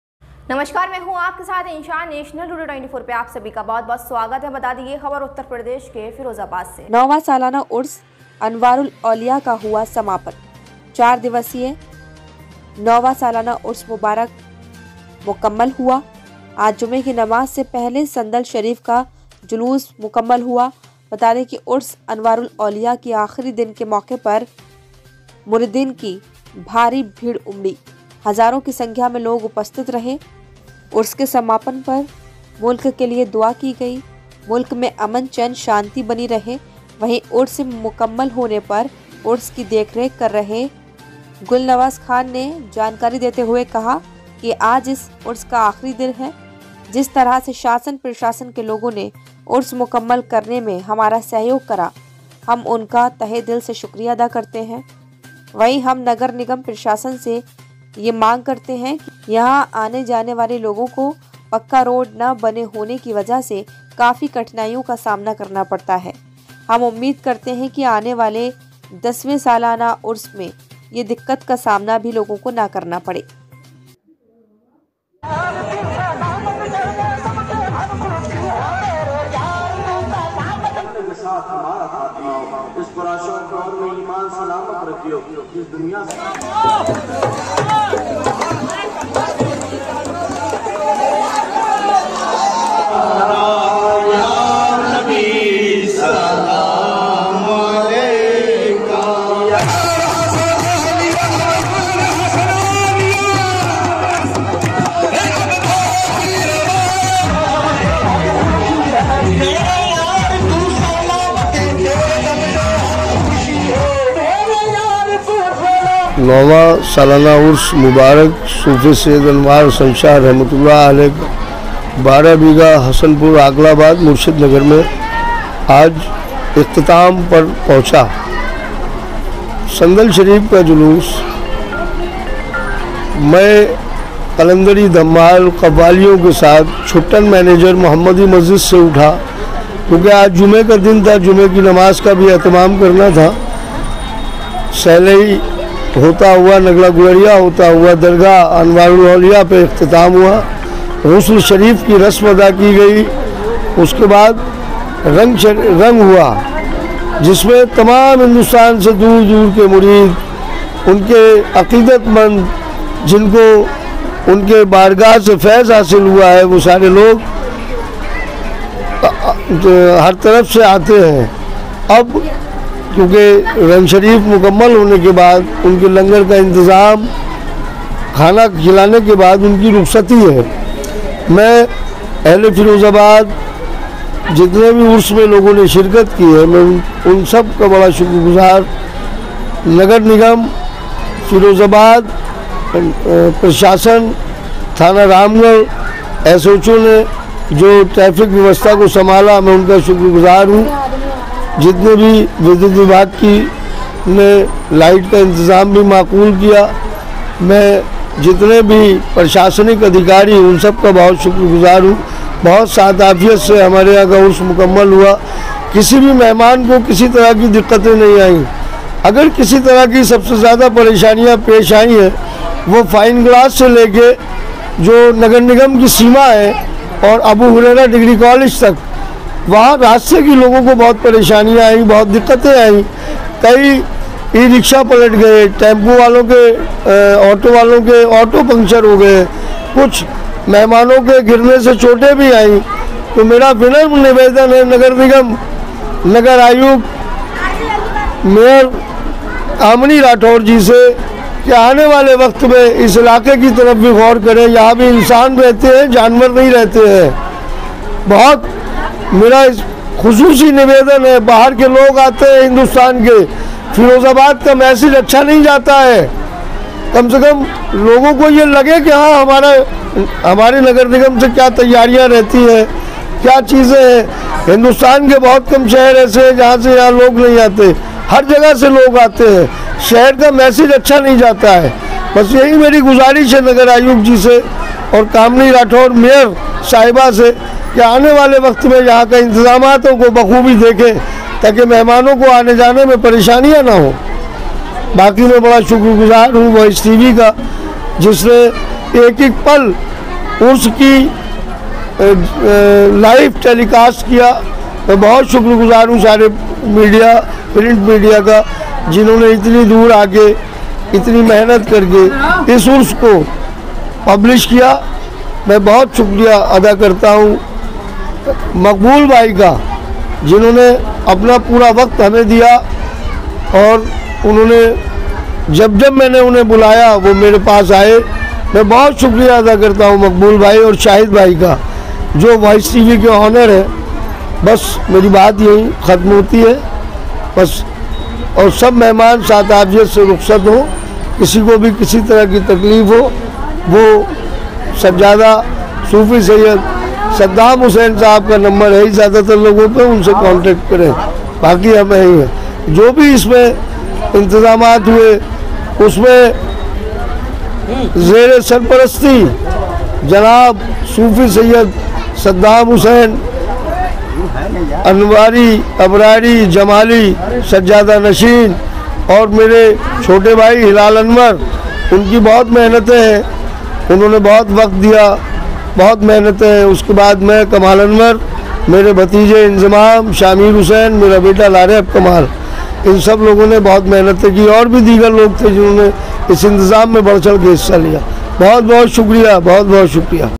9,वां सालाना उर्स अनवार उल औलिया का आज हुआ समापन, जुम्मे की नमाज से पहले जुलूस हुआ मुकम्मल
फ़िरोज़ाबाद में 9,वां सालाना उर्स अनवार उल औलिया का आज हुआ समापन, जुम्मे की नमाज से पहले संदल शरीफ का जुलूस हुआ मुकम्मल।